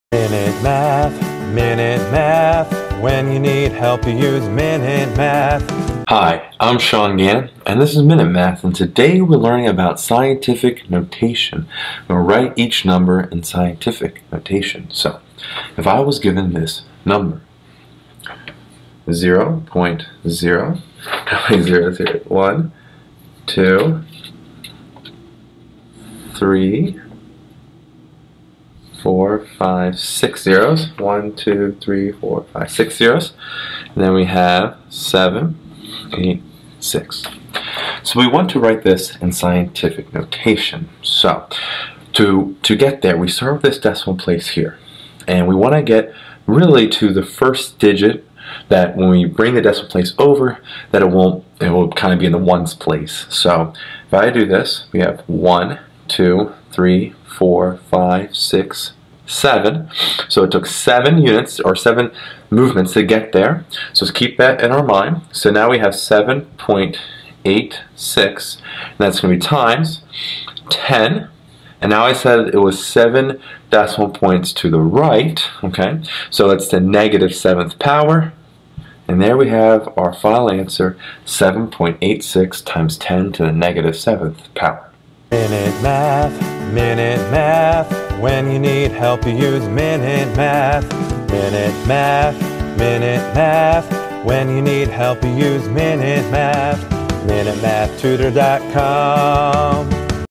Write in Scientific Notation: 0.000000786
In this math video lesson on Scientific Notation, I write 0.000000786 in scientific notation.